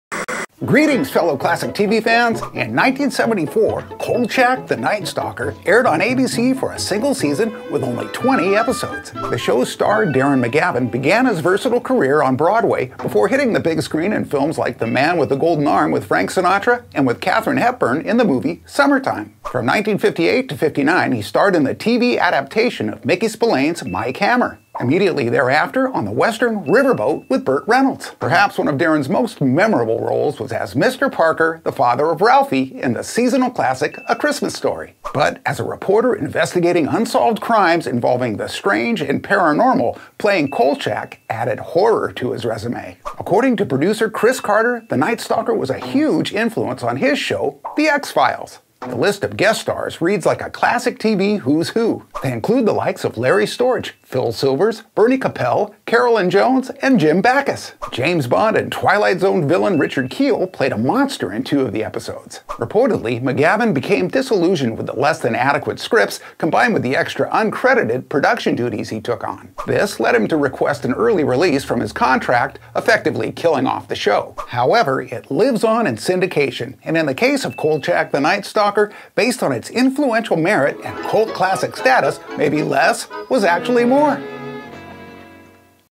KOLCHAK: THE NIGHT STALKER!
Darren McGavin rounds out his acting resume' with horror in Kolchak: The Night Stalker! Although it was a relatively short run, its' influence as a paranormal thriller ran deep. It even served as an influence for TheXFiles now it enjoys its status as a TV cult classic.